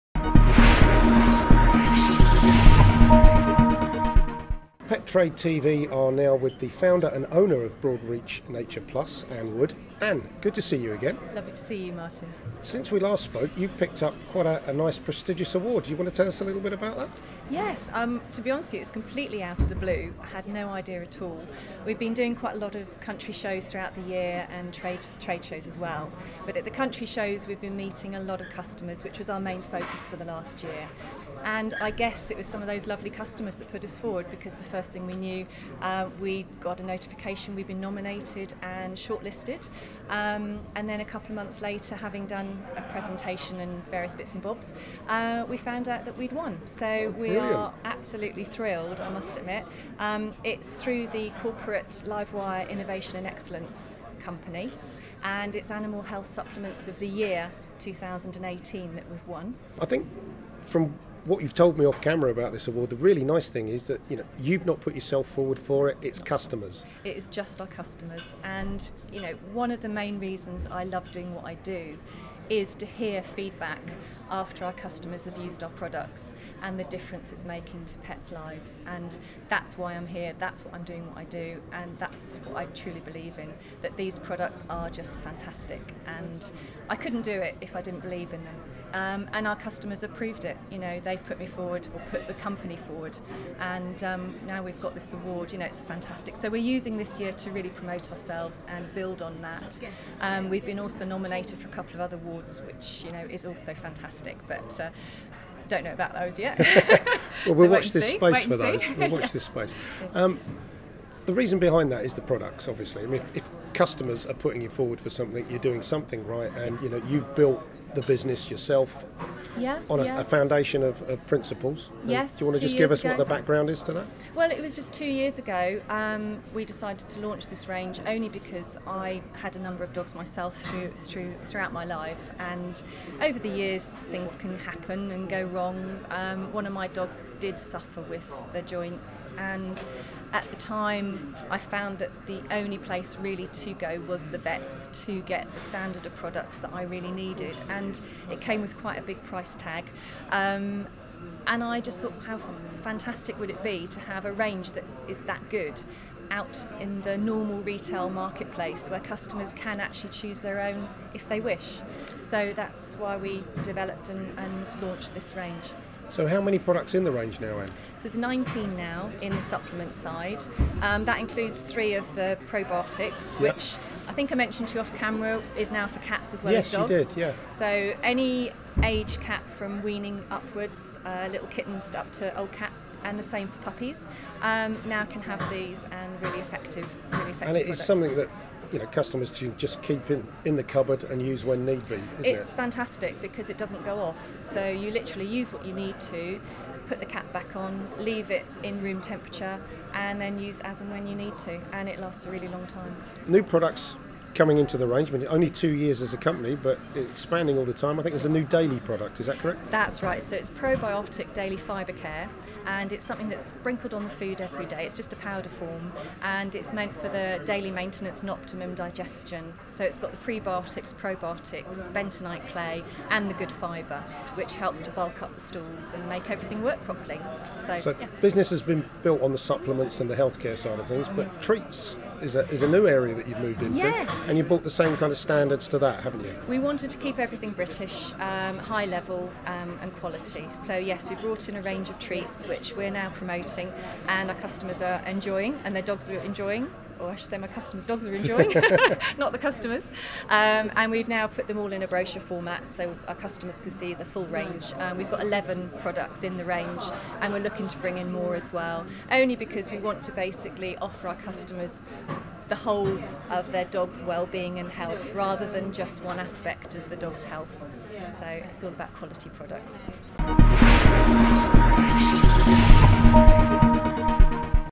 Anne of Broadreach Nature + Talks Awards & expanded product range
Anne, owner and founder of Broadreach Nature + talks to Pet TV about our most recent awards and advanced product range.